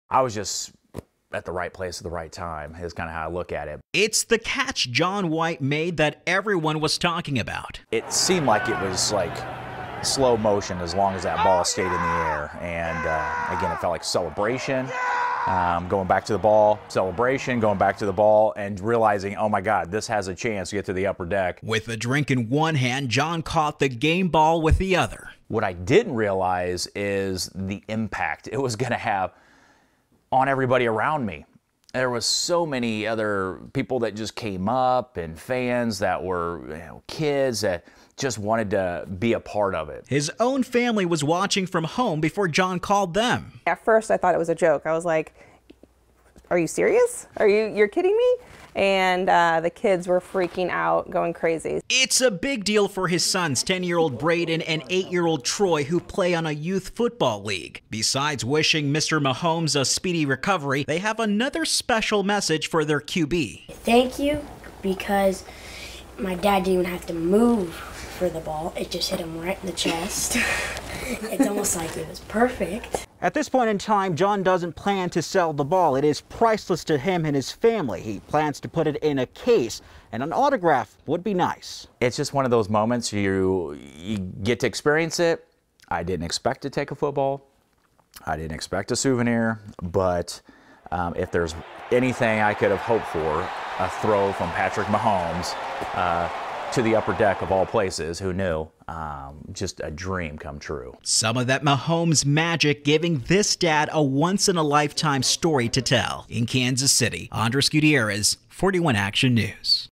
Chiefs fan elated over catching postseason game ball
It was the catch made by a Kansas City Chiefs fan that had almost everyone talking Sunday right after Chiefs all-star Patrick Mahomes scored the first touchdown of the postseason.